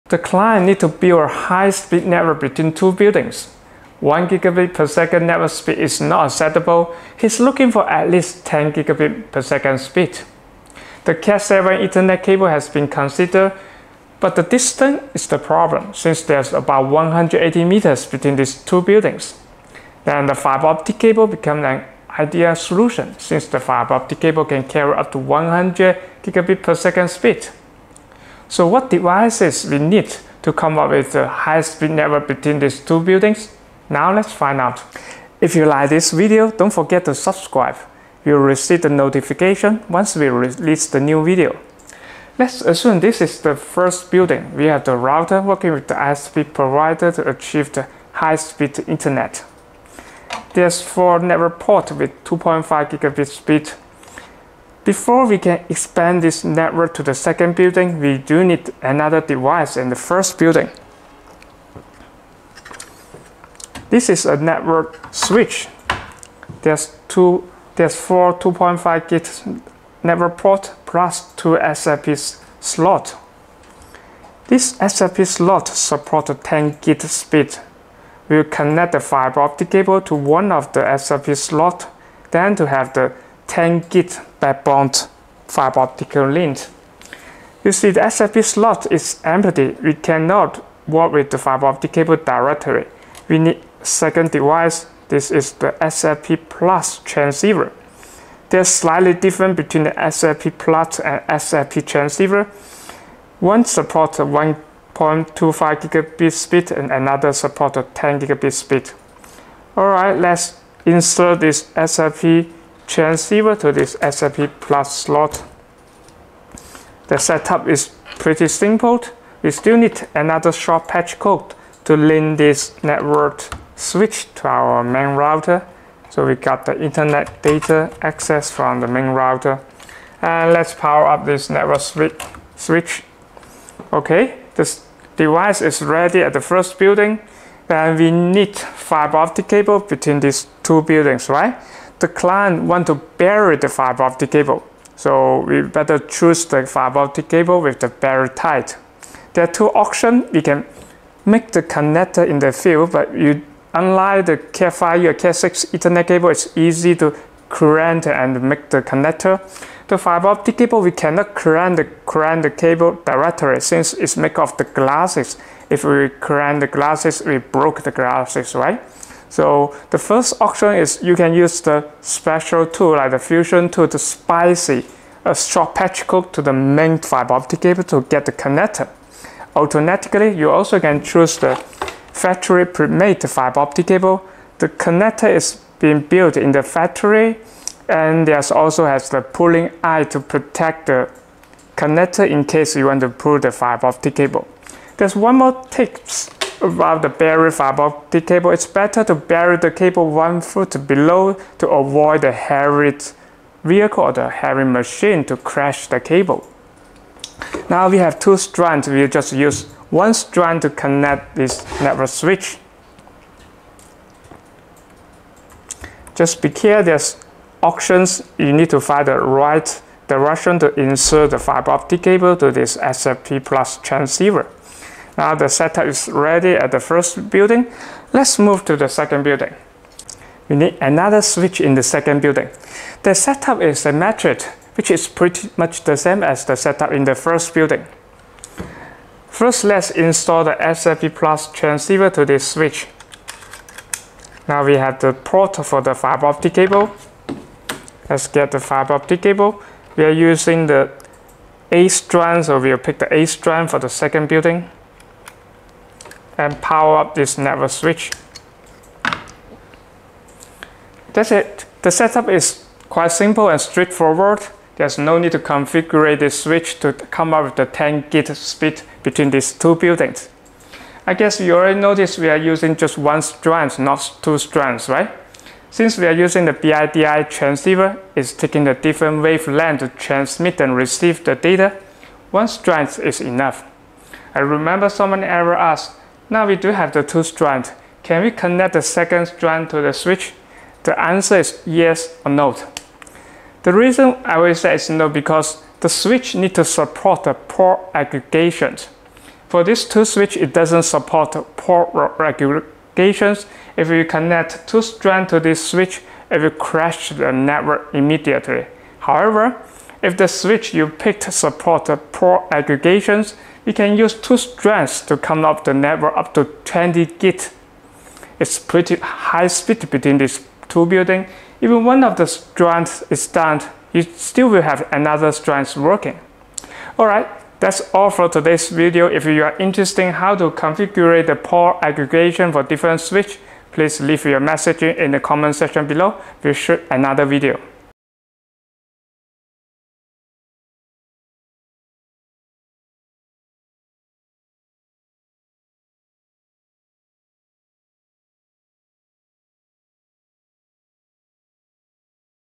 How to Set Up a 10Gbps Fiber Network Between Two Buildings | Easy Step-by-Step Guide
Need ultra-fast internet between two buildings? In this video, we walk you through how to build a 10Gbps high-speed fiber optic network from scratch! The client needed a network faster than 1Gbps across 180 meters, which ruled out standard CAT 7 Ethernet. Enter fiber optic cabling — capable of delivering speeds up to 100Gbps over long distances. Whether you're a networking pro or a DIY enthusiast, this setup is simple, reliable, and blazing fast. No need to configure switches manually — just plug and go!